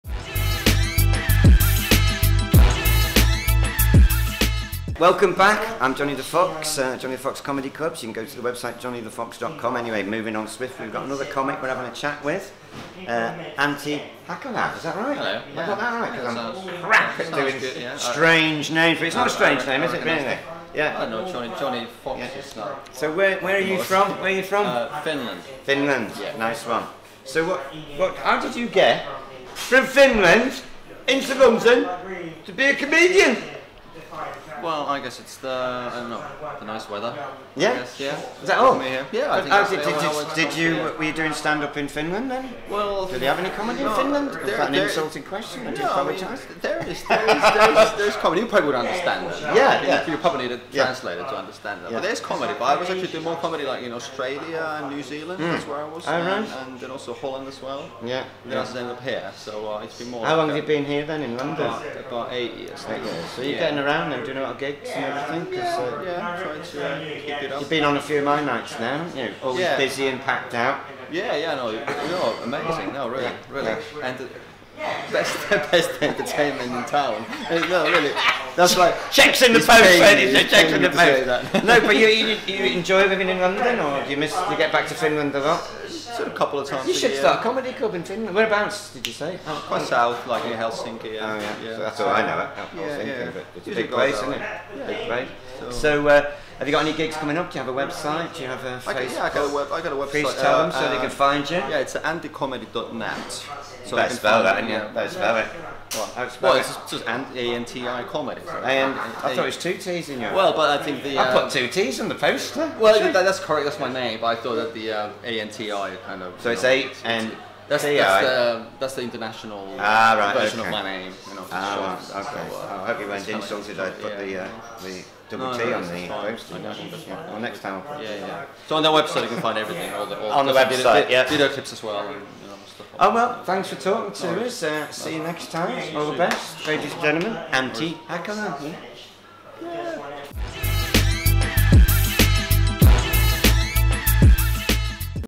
®️The Funny Circle - Comedy Vlog | Interview with Comedian - Antti Hakala | Finland
We had Jonny the Fox interview the hilarious Antti Hakala after his gig  at the Jonny The Fox Comedy night @The Crown & Anchor Pub in Covent Garden!